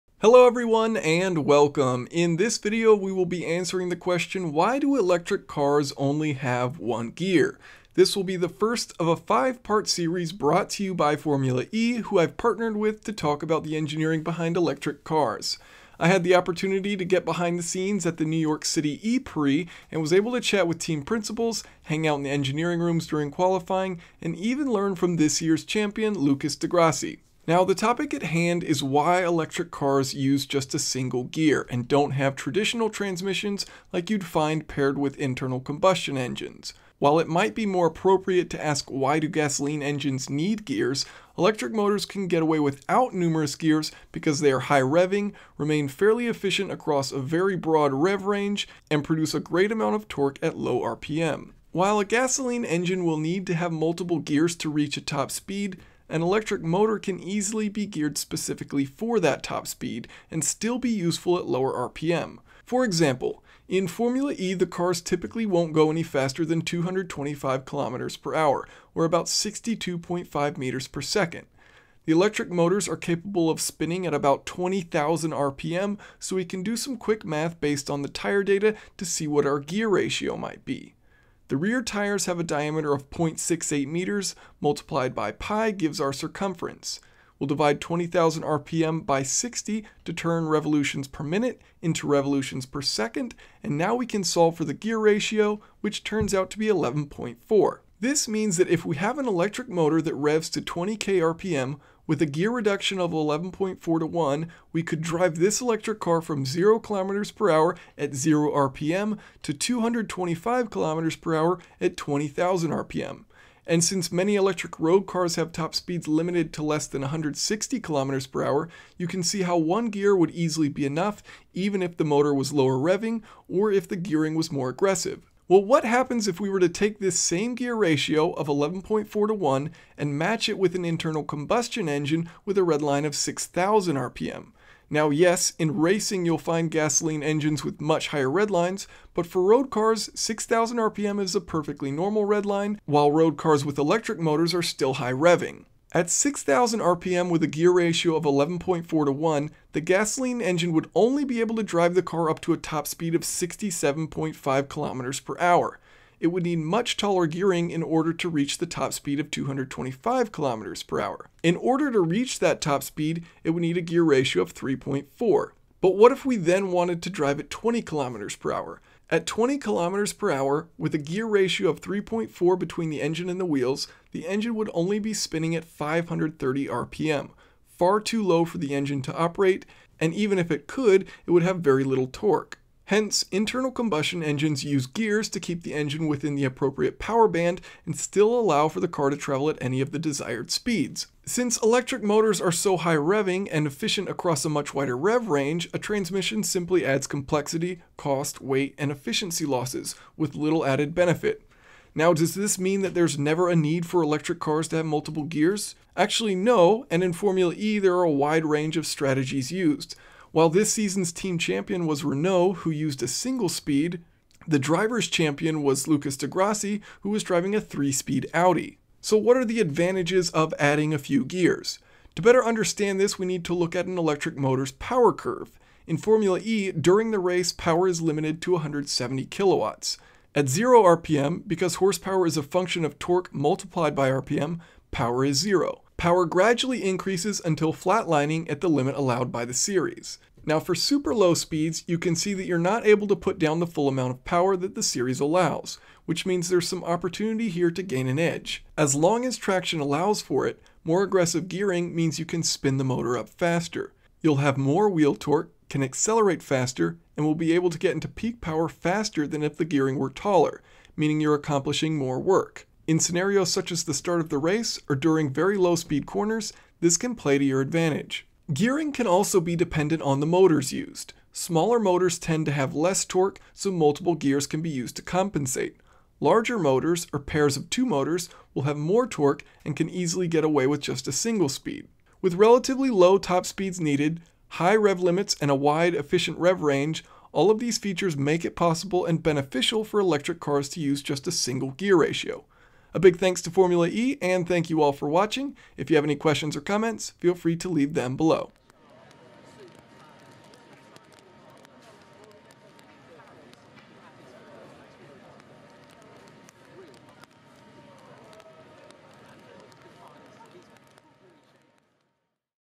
Why Do Electric Cars Only Have 1 Gear?
Why do electric cars only have 1 gear? I had the opportunity to get behind the scenes at the New York City E-Prix, and was able to chat with team principals, hang out in the engineering rooms during qualifying, and even learn from this year’s champion, Lucas di Grassi.

So why do electric cars use just a single gear, rather than using traditional transmissions like you’d find paired with internal combustion engines? Electric motors can get away without numerous gears because they are high revving, remain fairly efficient across a very broad rev range, and produce a great amount of torque at low RPM.